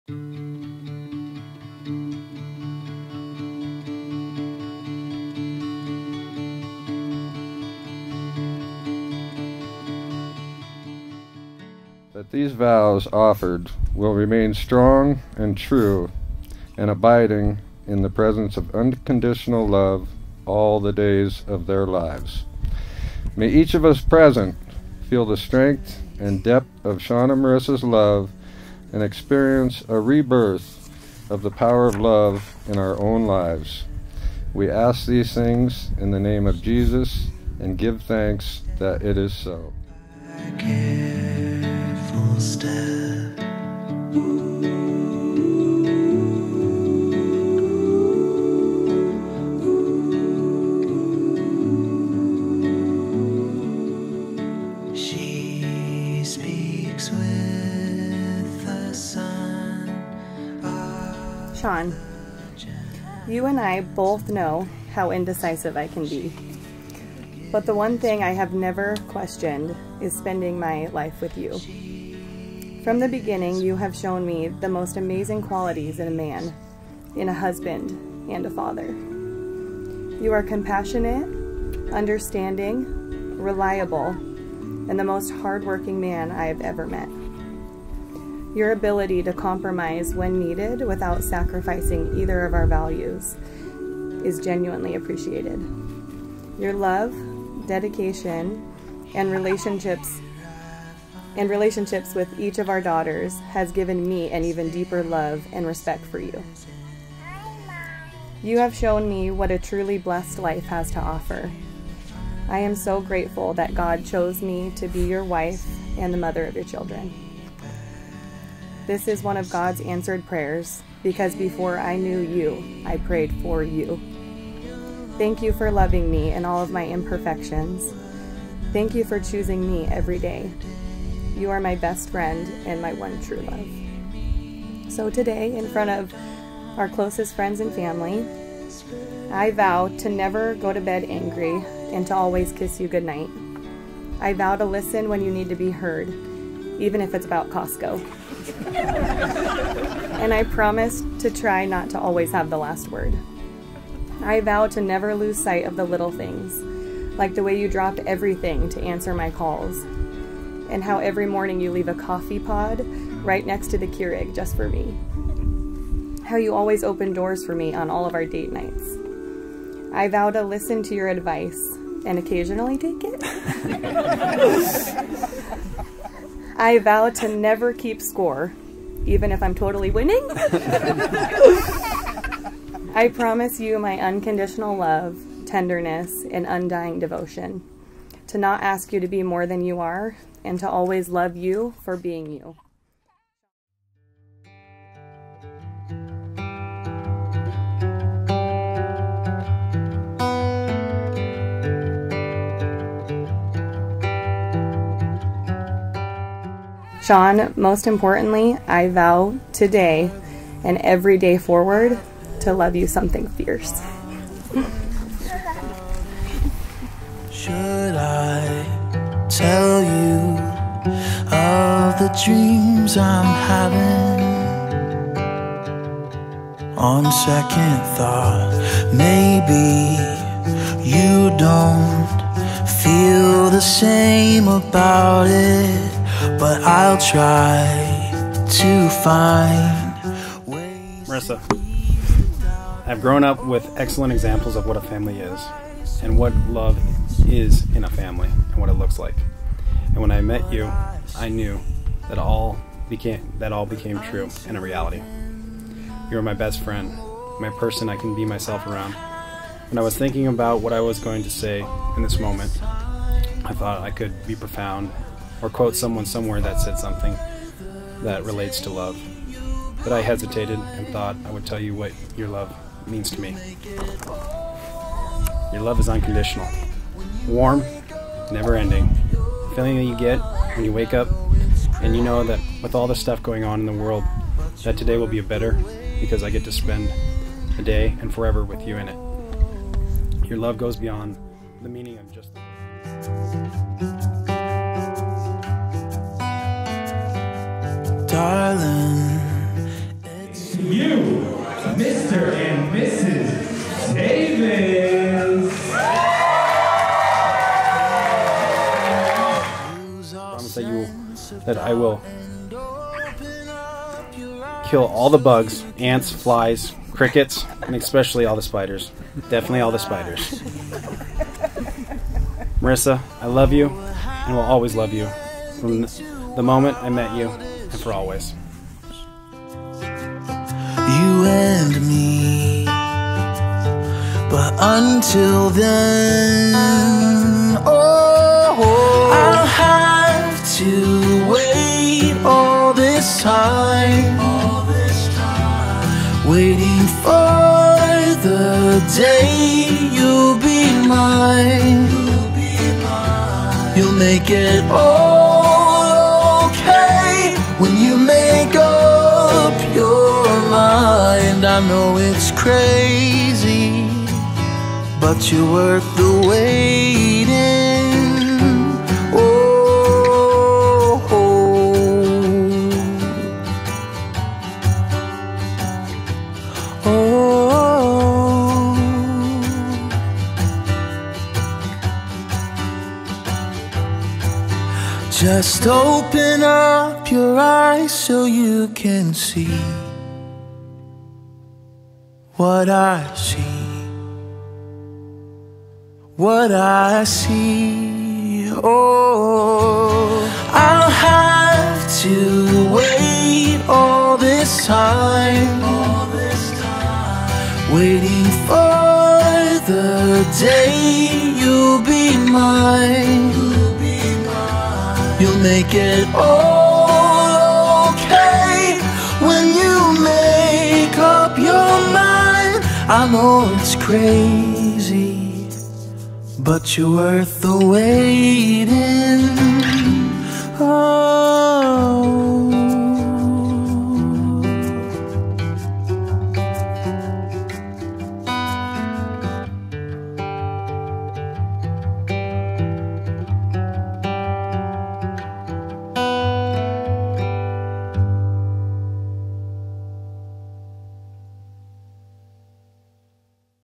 Whimsical Wedding Rugged Horizon Montana | Sean + Marissa | Infinite Photography and Film Missoula
Wedding of Sean and Marissa Davis at the beautiful Rugged Horizon, St. Ignatius, Montana.

Missoula Wedding Photographer | Montana Wedding Photographer
Missoula Wedding Videographer | Montana Wedding Videographer